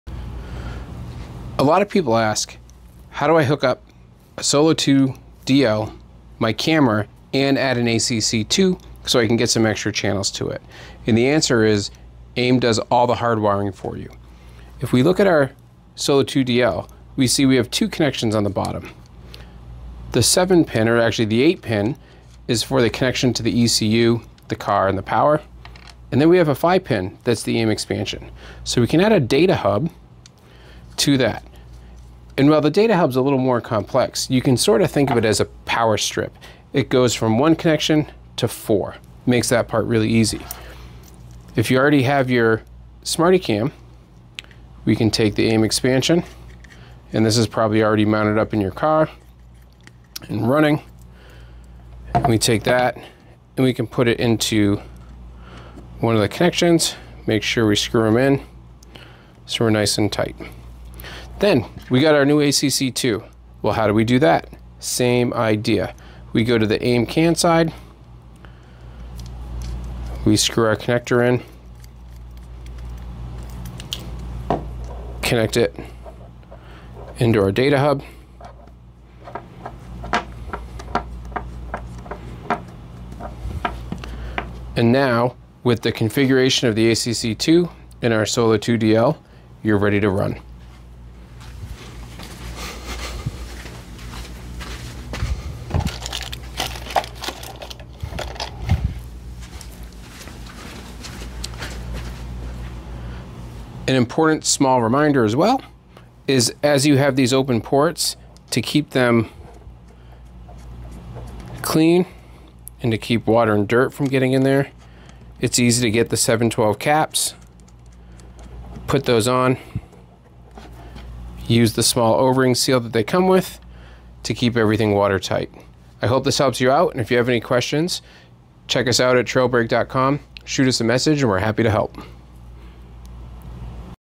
How to connect a Solo 2 DL to an ACC or ACC2
Confused about the wiring? Check out this quick video to see now  you can easily wire a ACC2 into your Solo 2 DL so you get up to 4 analog sensors!